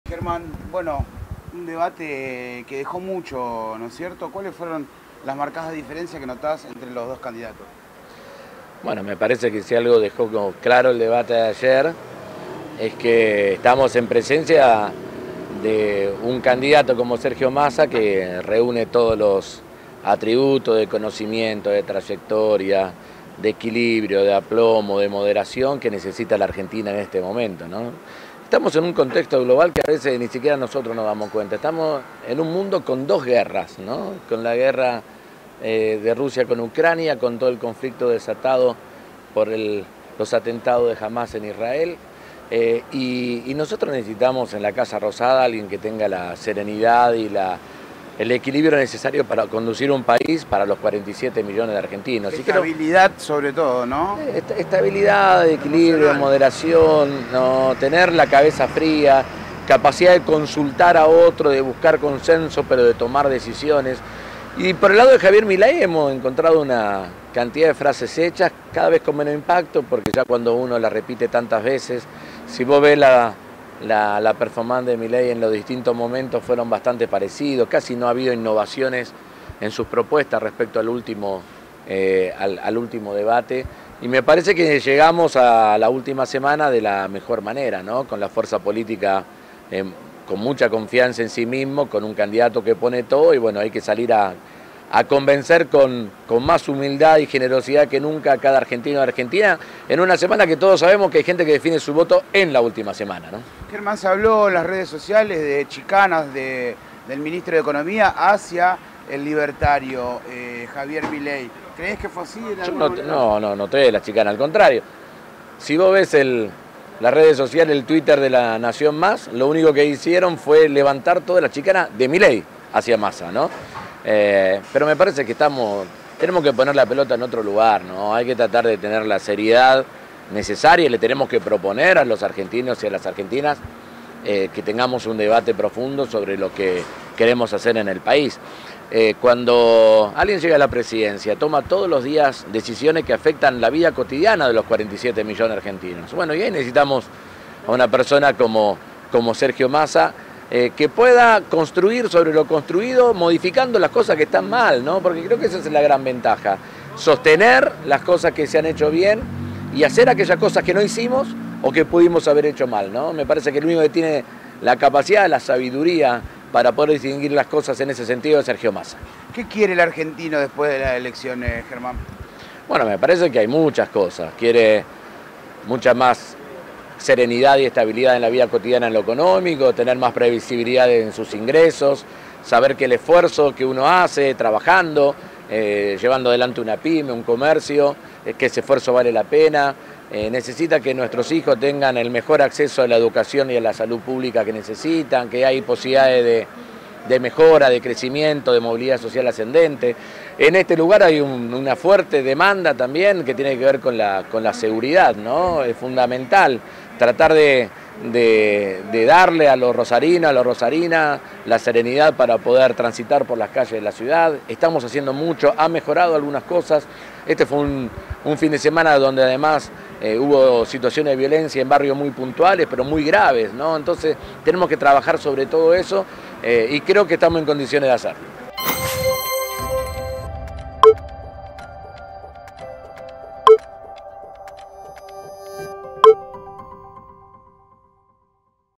Martínez: «Massa reúne todos los atributos que necesita Argentina en este momento»
El diputado nacional y presidente del bloque del Frente de Todos dio su parecer sobre lo que dejó el último debate presidencial, también se enfocó en lo que queda de campaña antes del balotaje y se coló el tema Rosario.